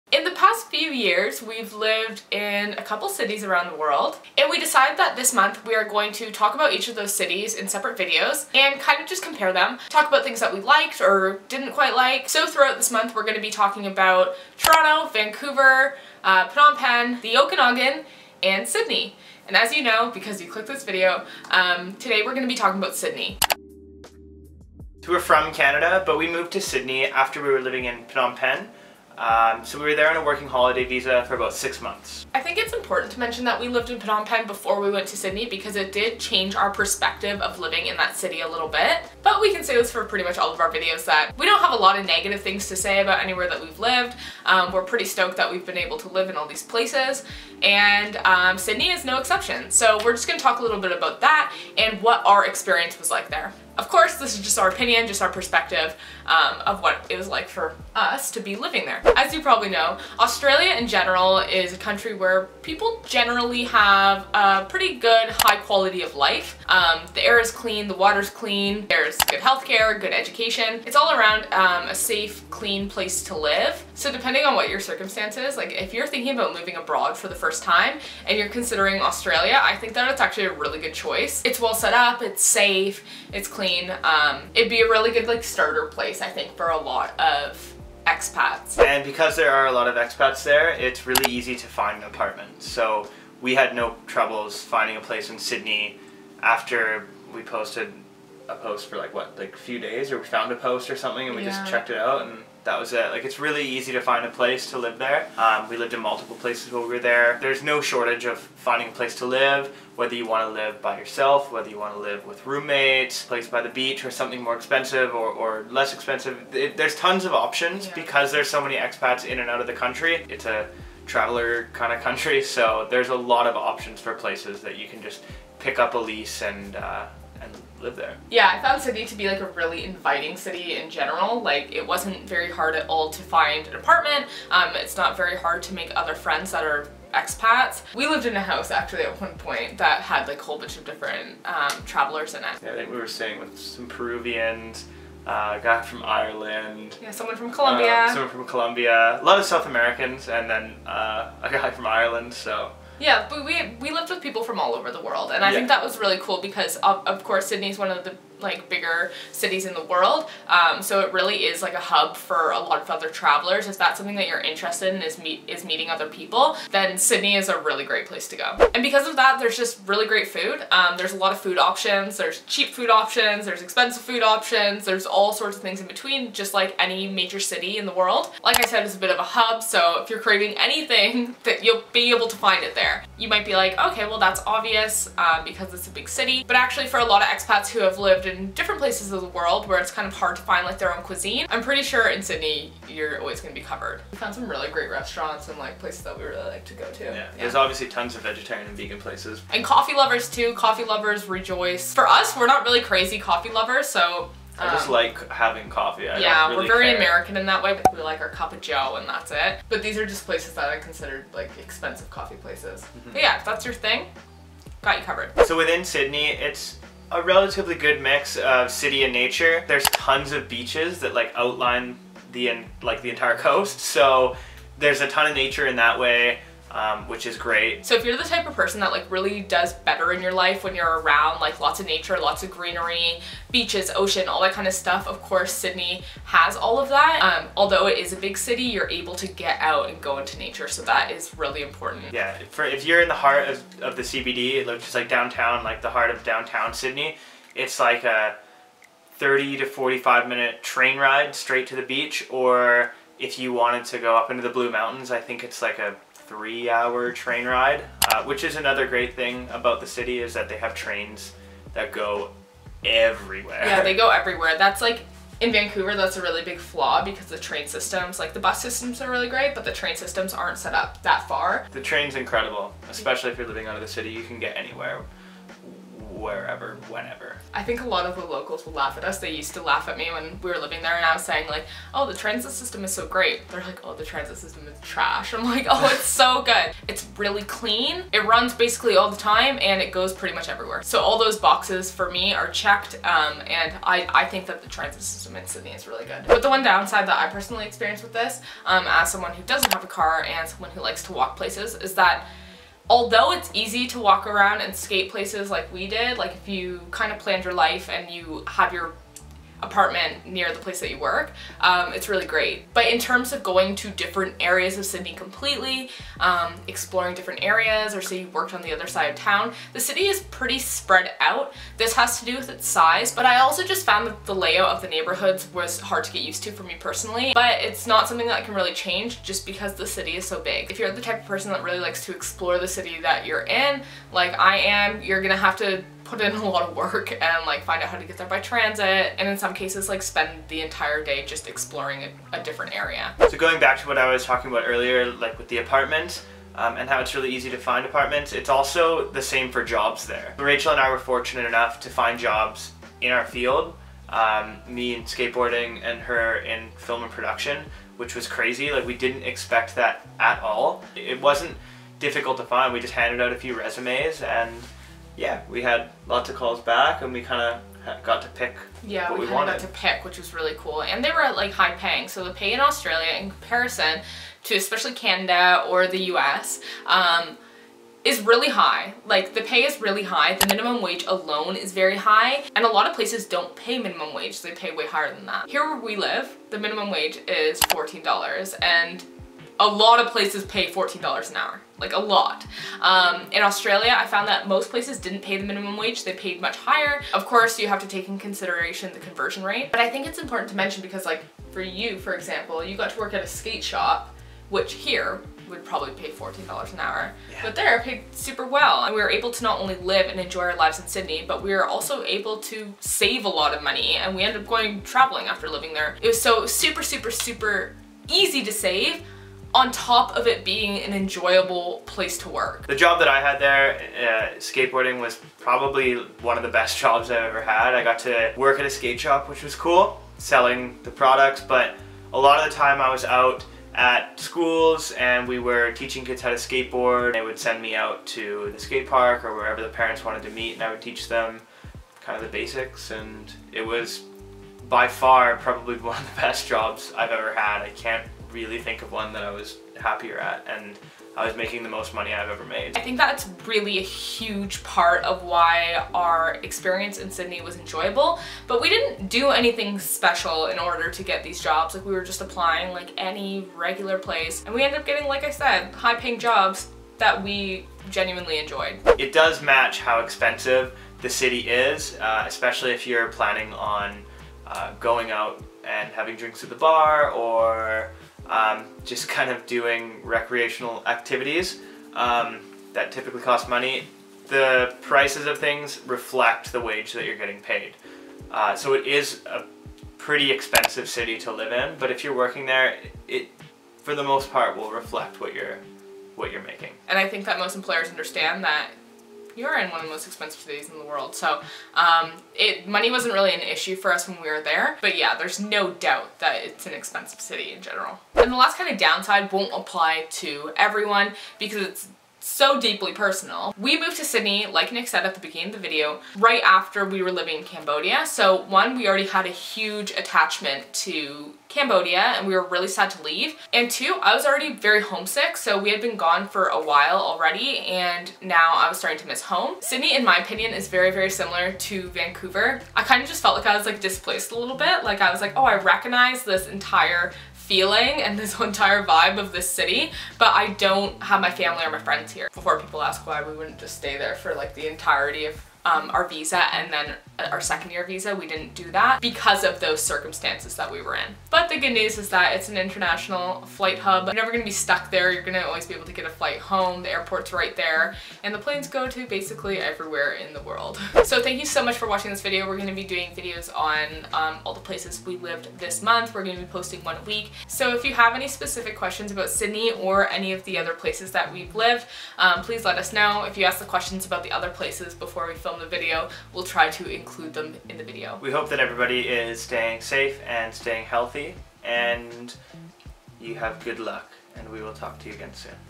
LIVING IN SYDNEY // Moving to Sydney, Australia on a Working Holiday Visa (Our Experience)
We talk about *our experience* living in Sydney, Australia - what it was like to move to Australia on a Working Holiday Visa, living in Sydney as an expat and moving to Sydney in general.

If you're wondering about how you can do a Working Holiday Visa in Australia, we have a whole video about that here!

What is it like living in Sydney, Australia? It's the first video of our series speaking on the places we've lived throughout the past few years. If you want to change your life and move abroad, live overseas, teach English, and/or travel, we are here for ya!